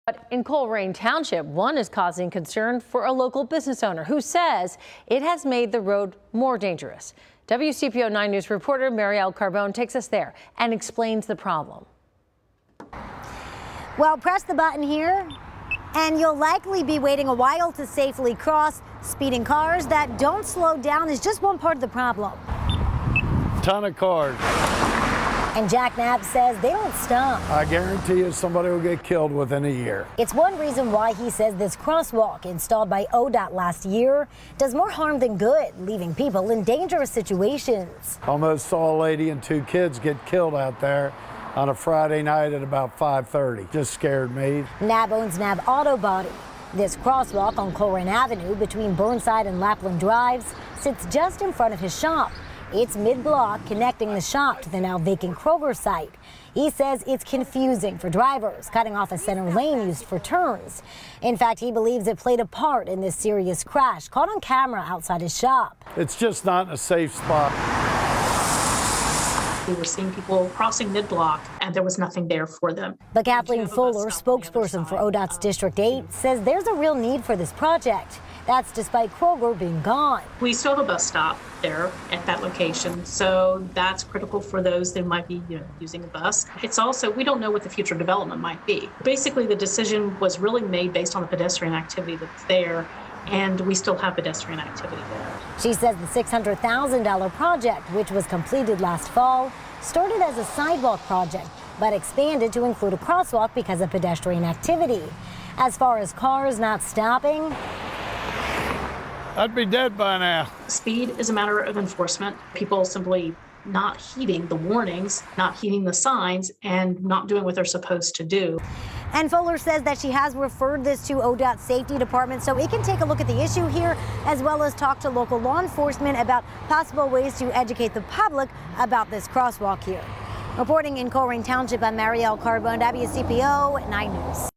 Colerain Township business owner concerned crosswalk does more harm than good
The crosswalk, installed by ODOT last fall, sits just in front of Knab’s shop on Colerain Avenue and connects to where the vacant Kroger site is. Without the Kroger store, Knab believes there is no reason for the crosswalk to still exist.